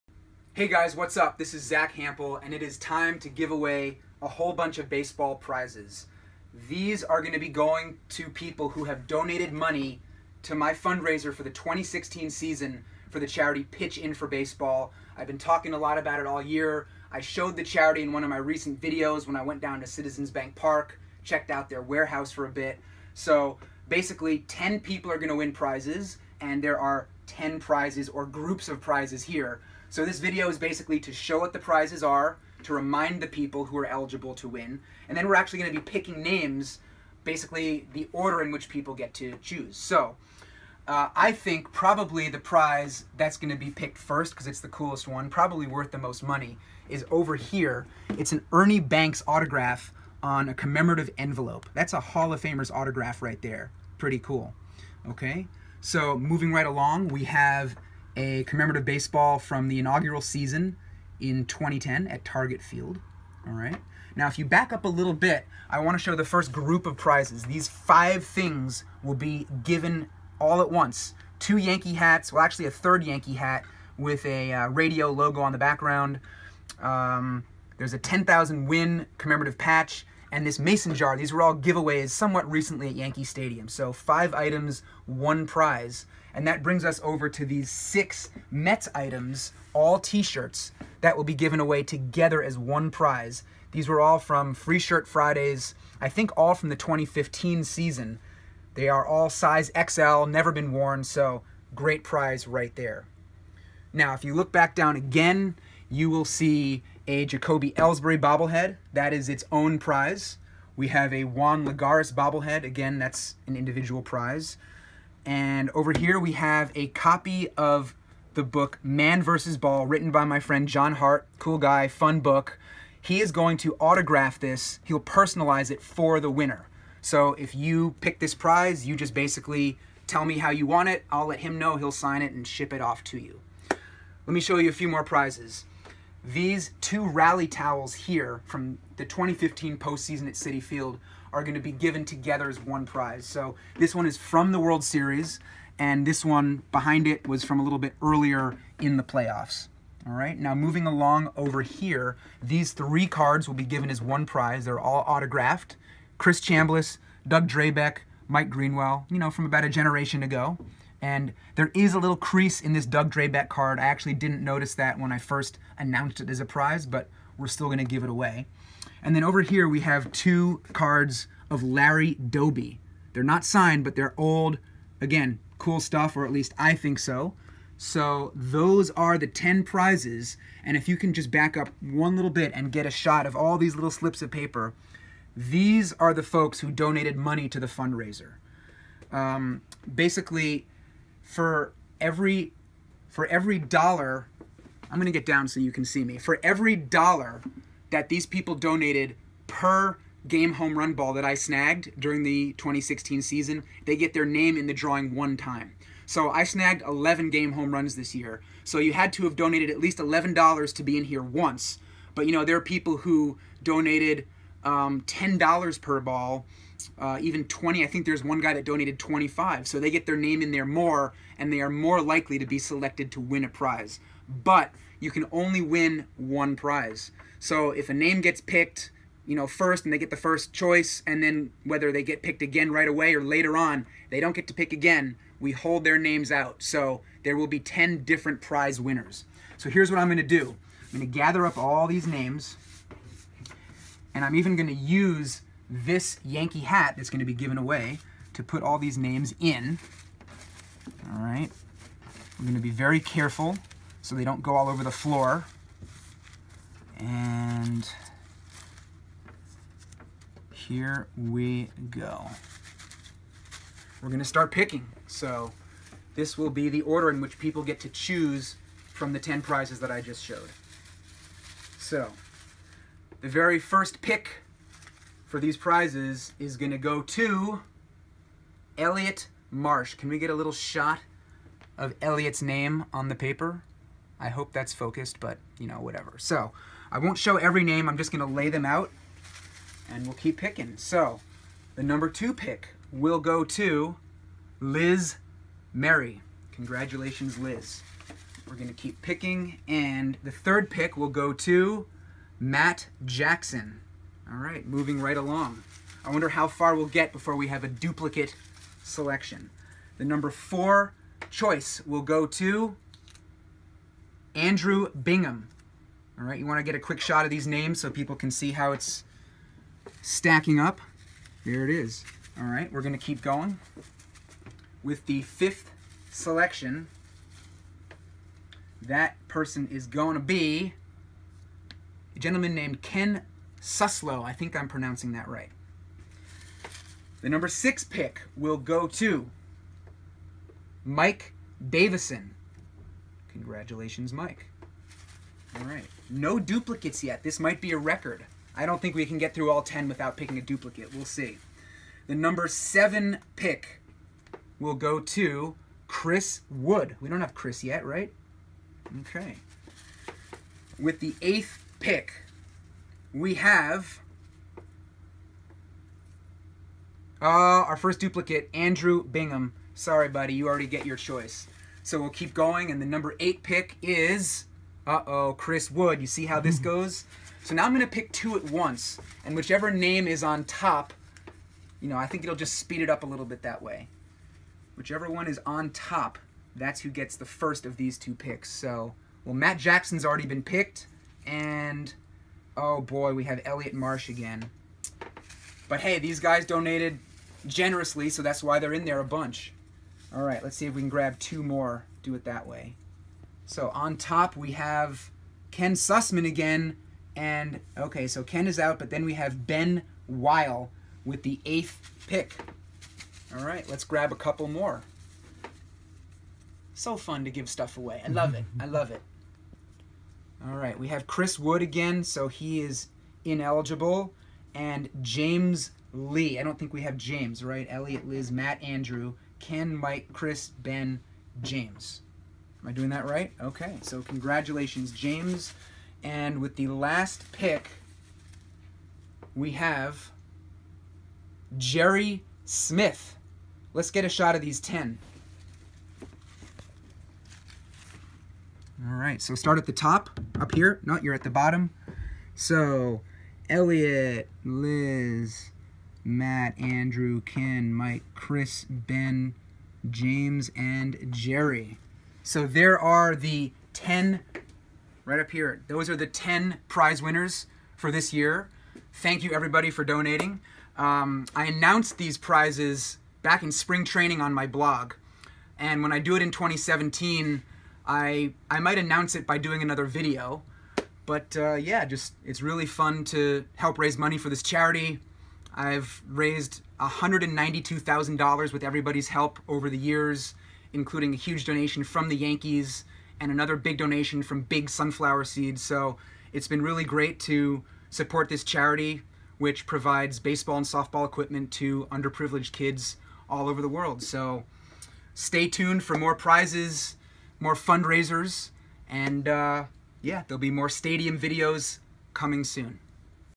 Charity prize drawing 2016
Every season since 2009, I've been doing fundraisers to benefit the charity Pitch In For Baseball (pifb.org). People who donated this year were eligible to win one of these prizes. This video shows the drawing -- just a fun way to announce the winners and spread the word and hopefully get more folks involved next year.